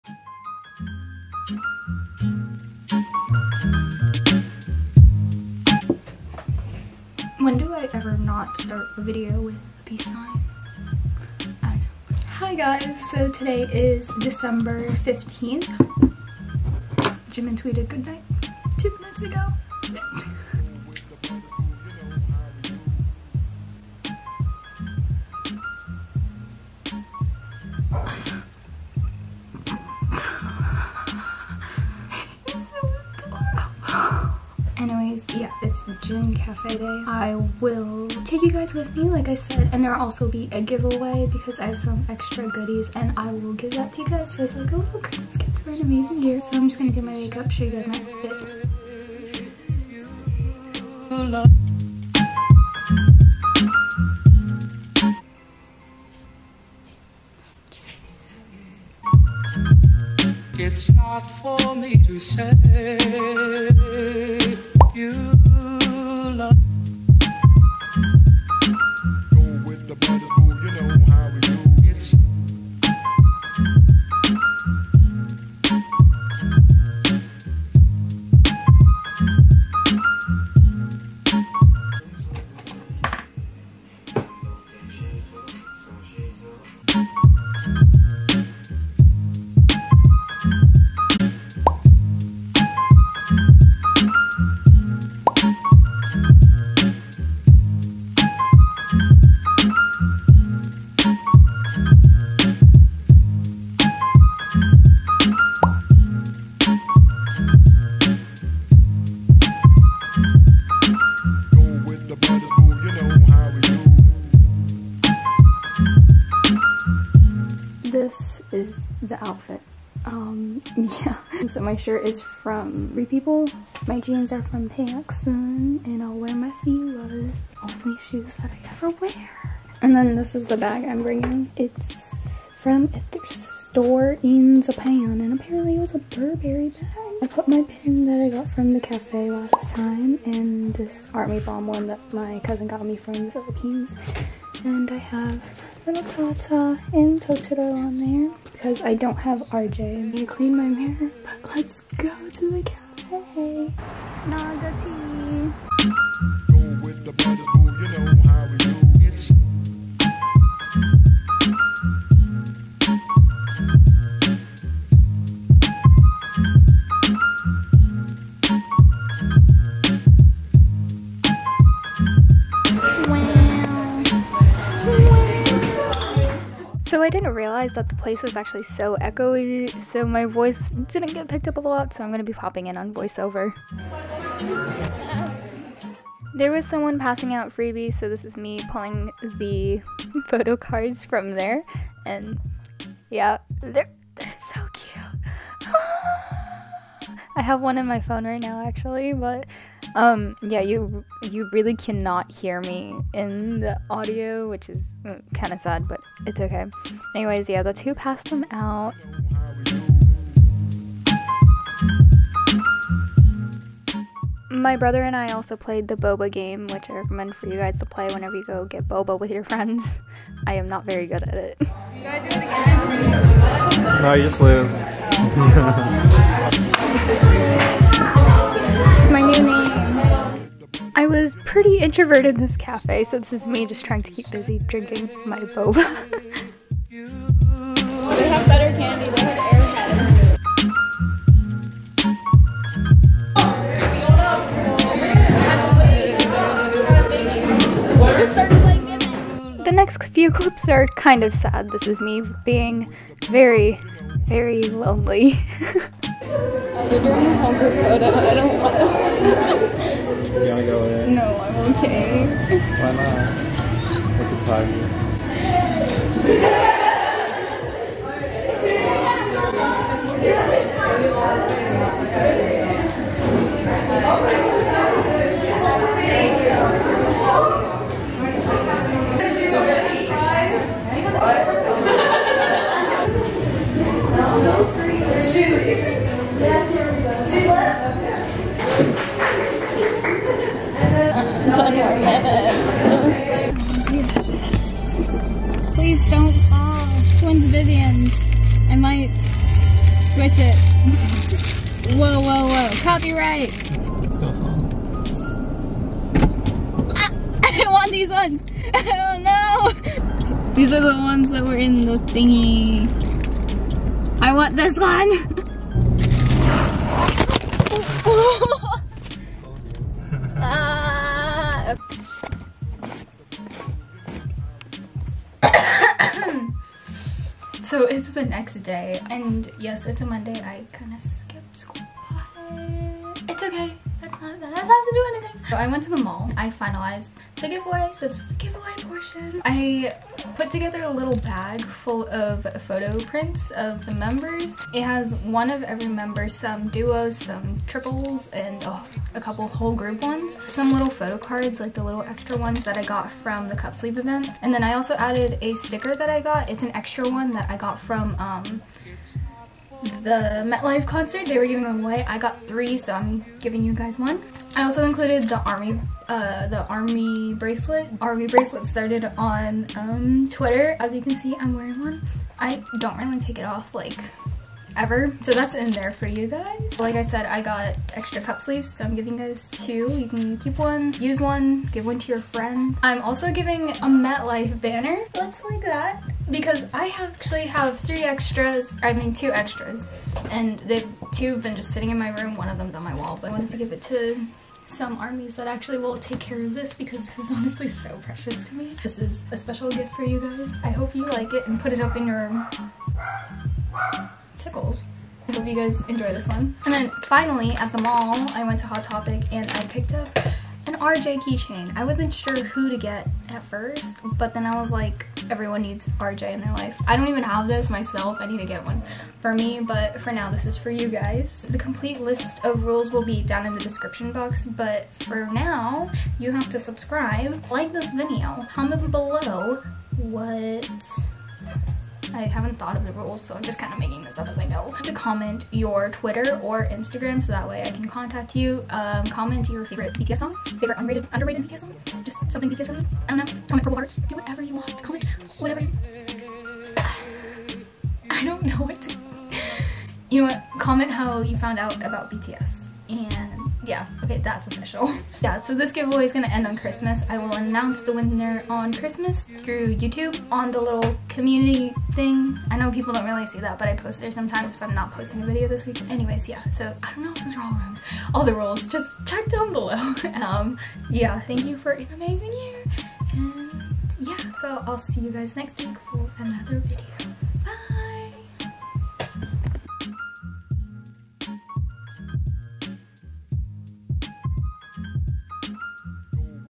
jin cup sleeve event + GIVEAWAY!!!
giveaway rules below!! ty for an amazing year and i just wanted to give a little something back to you guys because this was my best, most consistent year on youtube lol so THANK YOU!! PURPLE HEART EXPLOSION!!!

RULES (limited to US & Canada residents only):
3. tell me your favorite bts song!!
thats it! thank you thank you thank you from the bottom of my heart for everything!! i will try to do more giveaways in the future but rn im only a small channel but i hope we can grow into a community :))

V●ᴥ●V